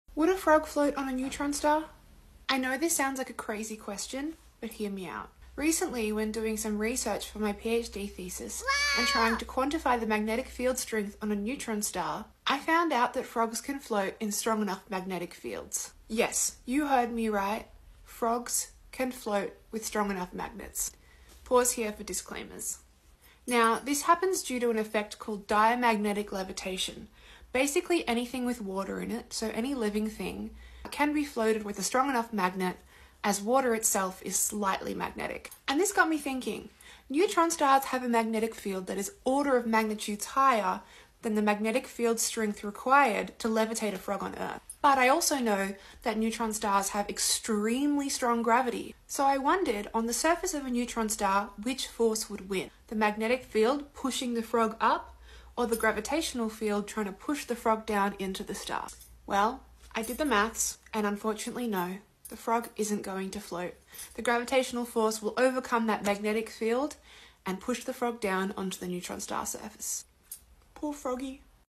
floating frogs on neutron stars!?
Do frogs float on neutron stars? 🐸 ⭐️ Join me for an exploration of the fight between magnetism and gravity on neutron stars 💫 **no frogs were harmed in the making of this video** space universe astro astronomy astrophysics science sciencetok STEM phd phdlife womeinstem @ozgrav